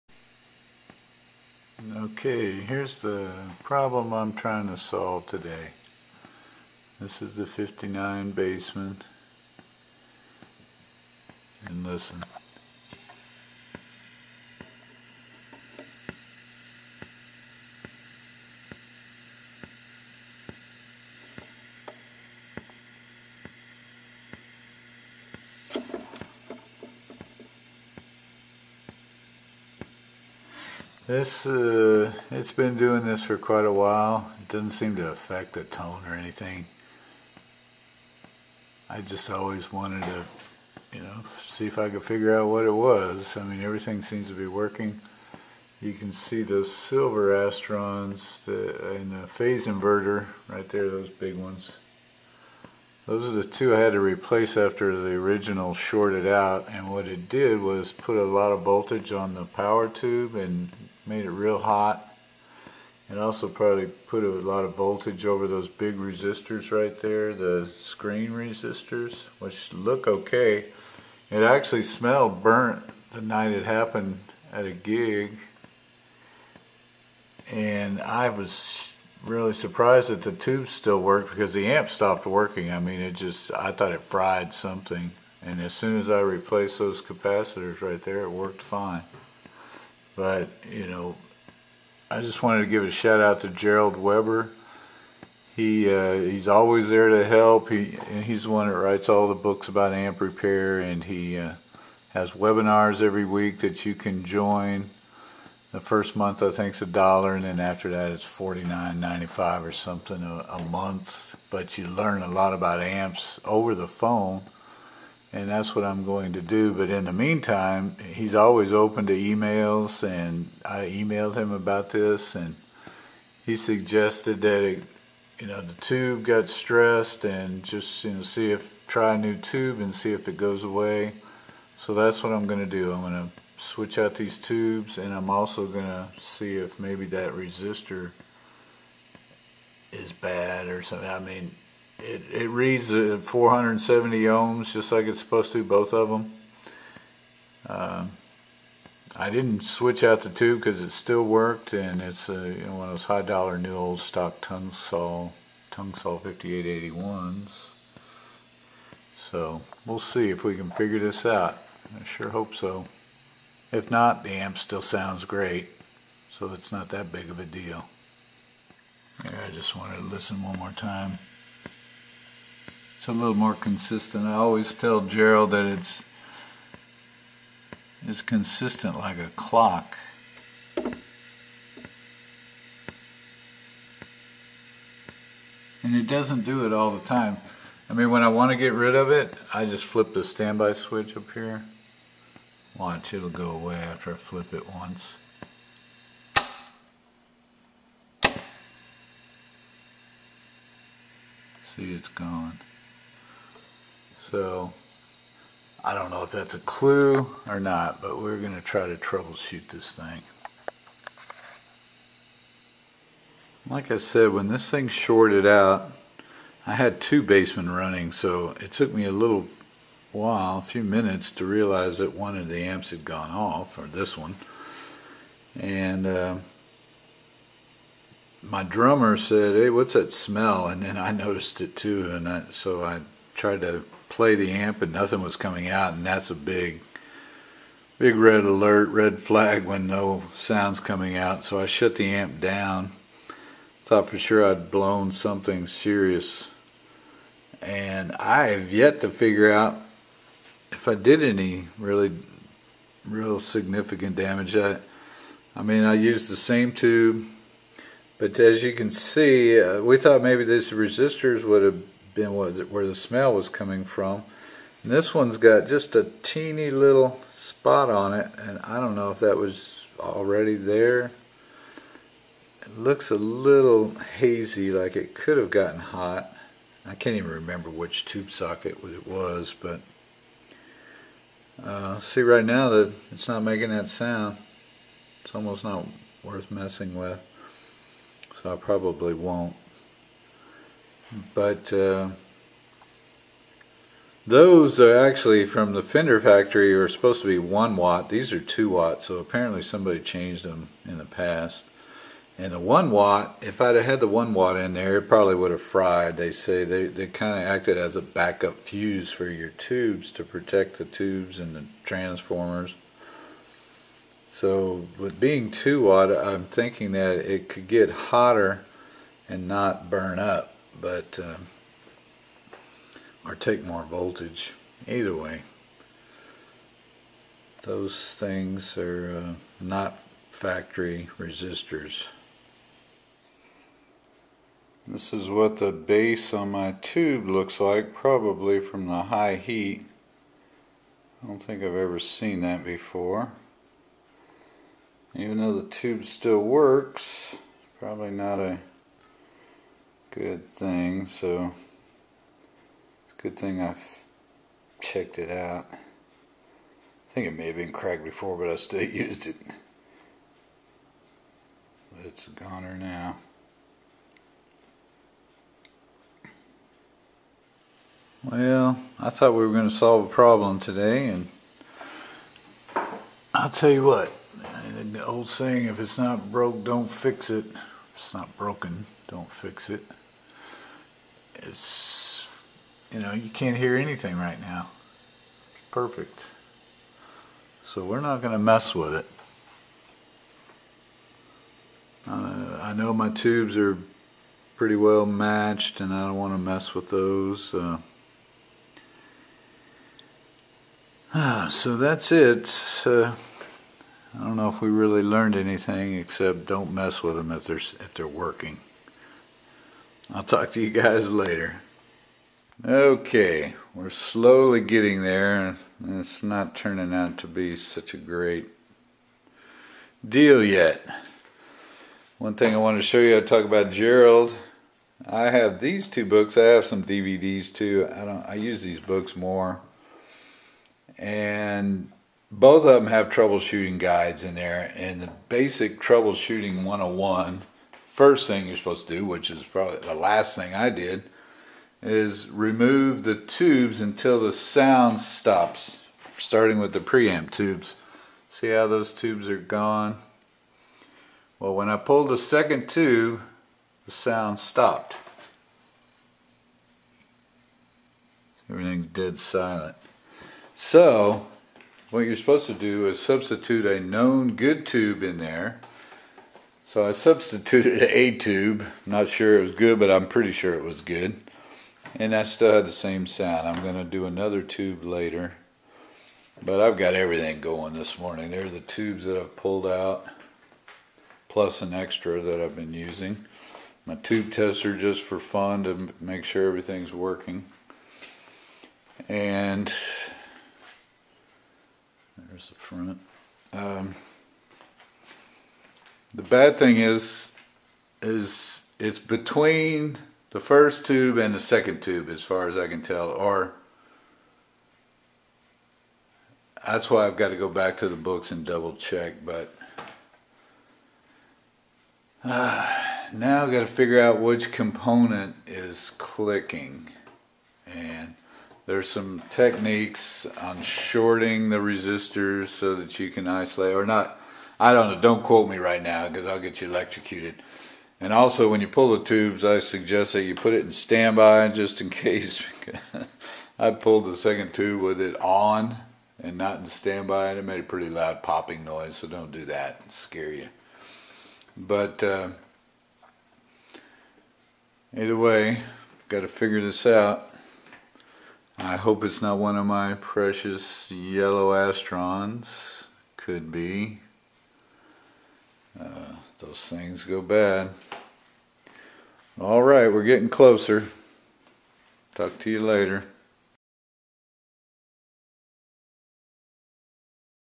Gerald Weber Helps Troubleshoot My Vintage 1959 Bassman
This is mainly a shout-out to Gerald Weber who has always been there in my time of amp crises over the past nine years.  And also to Dan at Area 51.  They have always been there to help when I couldn't figure things out with my amps.  I appreciate all of the help.  Gerald's books are referenced in this video.  I use them to troubleshoot my amps and answers are just an email away if I can't find them in the books.  Gerald has a weekly webinar subscription if you are interested in learning about amp design and maintenance.  I also went to Gerald's Amp Camp in Texas years ago which was a lot of fun.  I never quite pinpointed the problem of the ticking noise but have put it on hold for now.  We will discuss the issue further at next week's webinar.  The amp sounds wonderful especially now that I have changed out the power tubes again to a better pair.  You see that one of my old tubes may have cracked from overheating when a capacitor in the phase inverter section of the amp shorted out at a gig and hit the 5881 with approximately 250 DC volts on the grid.  The grid should only see the amplified AC signal from the guitar.  It got very hot and it smelled like something was burning.  There are troubleshooting guides in most of Gerald's books.

Here is a link if you ever want to sign up for Gerald's weekly webinar

***Update*** Well, it turned out that it may have been the tube in V3.  I removed V2 and the sound stopped so I assumed it was the V2 tube but then it started again.  So removed V3 and it stopped.  I have since put in a NOS tube (a real Mullard made in Great Britain) in the phase inverter (V3) and it has not come back.  The sad part is that the V3 tube was the only new production tube I have used in my amps.  I use all NOS or slightly used NOS tubes in all of my amps.  It was a new Groove Tube Mullard Reissue tube made in Russia that was causing the noise.  This is exactly why I have shunned away from new tubes from Russia and China.  There are a lot of bad ones out there.  If anything changes, I will update it here.

On that note, Amp Maintenance 101 always emphasizes that 99% of all amp problems are tube related.